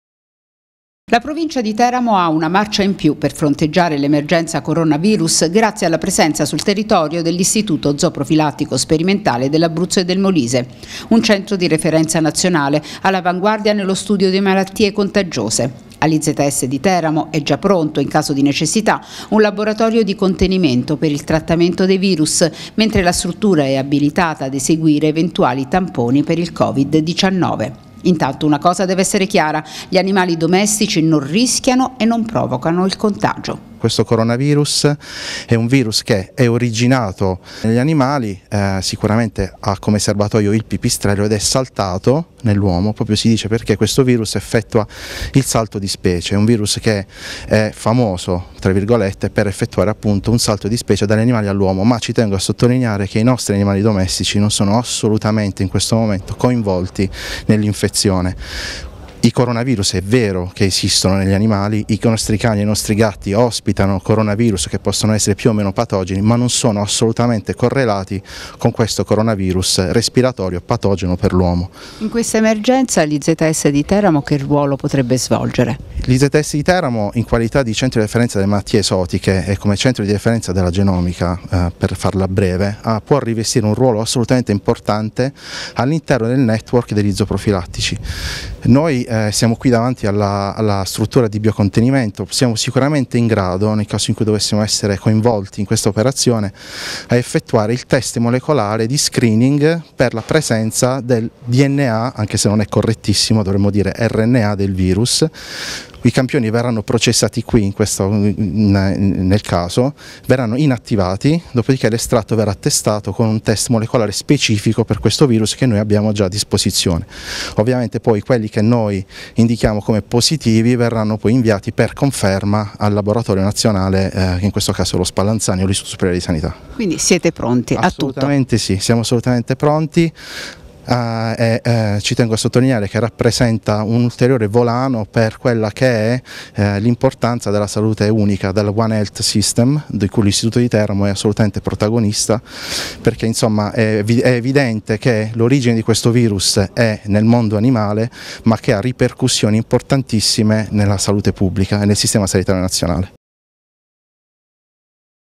Coronavirus: all'Izs è già pronto un laboratorio di contenimento
All'Istituto Zooprofilattico di Teramo è tutto pronto per fronteggiare l'emergenza da Coronavirus. In caso di necessità la struttura potrà eseguire i tamponi per il Covid19 ed è già disponibile un laboratorio di  bio-contenimento per il trattamento del virus